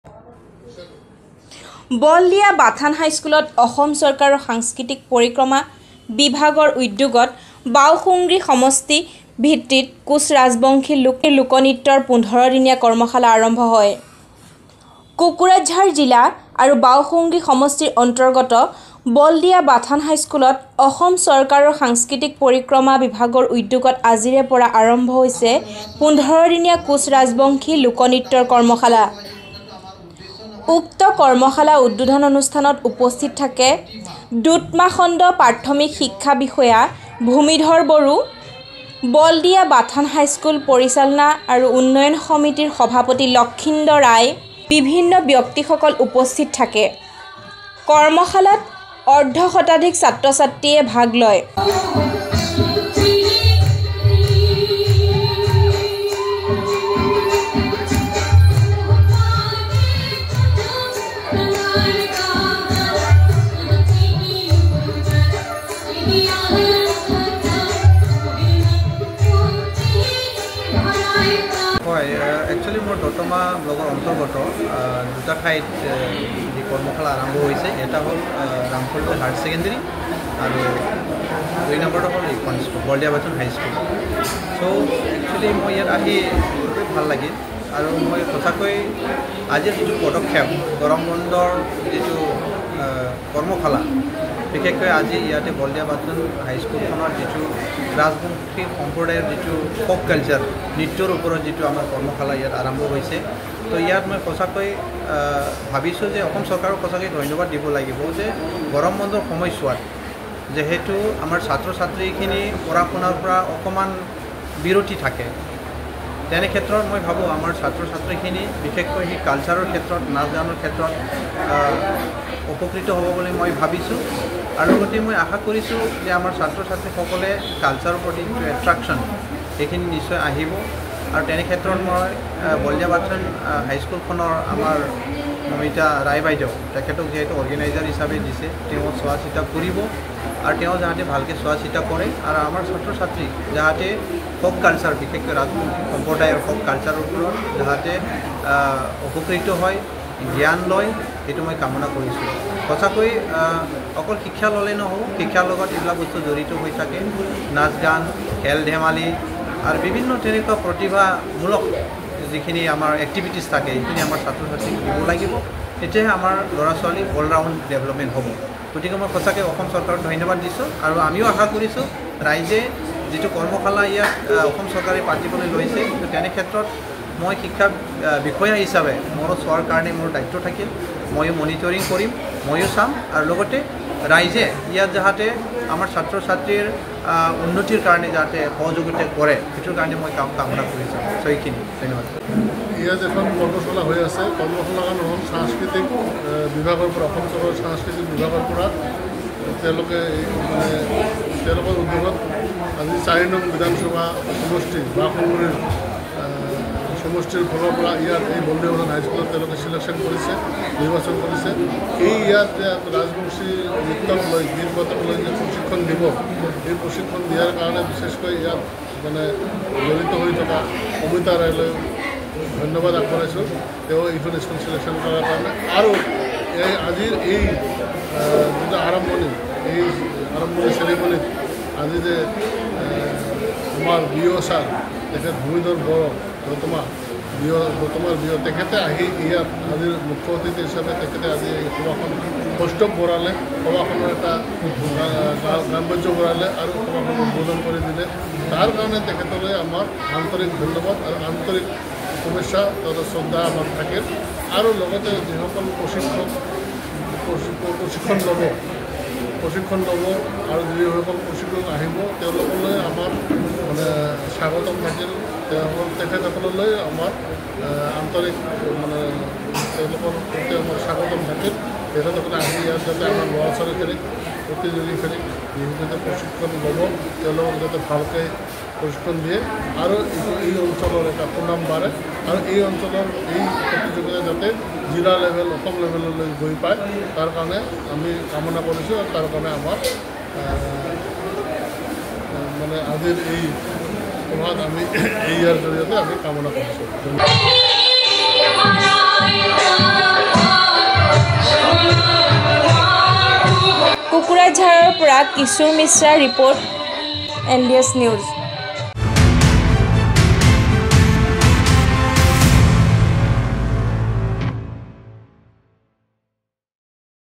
15-day workshop on Koch Rajbongshi folk dance at Baldia Bathan High School, Kokrajhar
15-day workshop on Koch Rajbongshi folk dance on Baokhungri basis started at Baldia Bathan High School under the aegis of Cultural Affairs Department, Government of Assam

A fifteen-day-long workshop on Koch Rajbongshi folk dance at Baldia Bathan High School under Kokrajhar district and Baokhungri constituency, under the aegis of Cultural Affairs Department, Government of Assam, has started.

 Dotama Khand Primary Education Officer Bhumidhar Boro, president of Baldia Bathan High School Management and Development Committee Laxmindra Ray, associate professor of SYG College Dr. Tarun Chandra Ray, Shakti Ashram VCDC chairman Meena Ray Barman, teachers Amaleswar Ray, Manik Kumar Ray and others were present on the occasion. More than hundred students participated in the workshop.